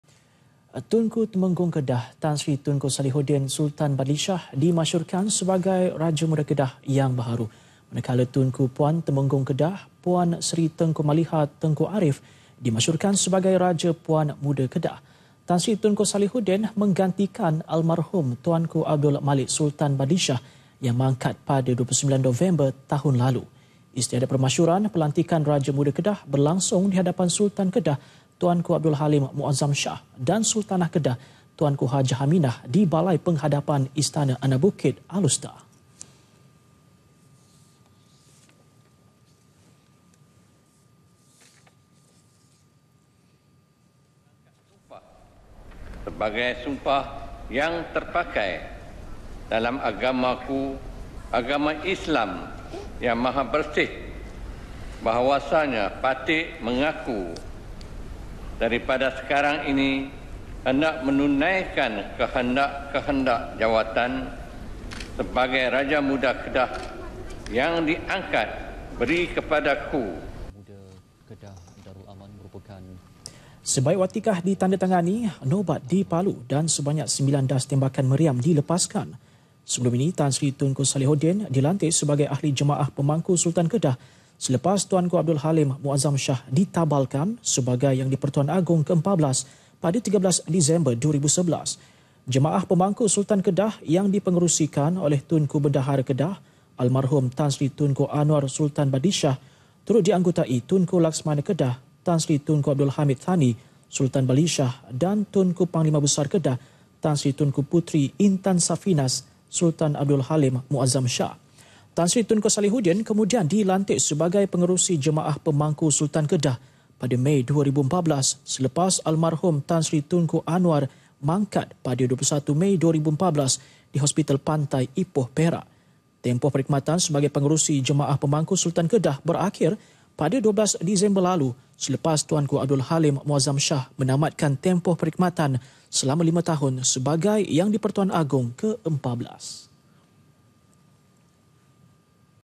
TUNKU TEMENGGONG KEDAH DIMASYHUR SEBAGAI RAJA MUDA KEDAH [15 DIS 2016]
Tunku Temenggong Kedah, Tan Sri Tunku Sallehuddin Sultan Badlishah dimasyhurkan sebagai Raja Muda Kedah yang baharu.

Manakala Tunku Puan Temenggong Kedah, Puan Seri Tengku Maliha Tengku Ariff dimasyhurkan sebagai Raja Puan Muda Kedah.

Tan Sri Tunku Sallehuddin menggantikan Almarhum Tuanku Abdul Malik Sultan Badlishah yang mangkat pada 29 November tahun lalu.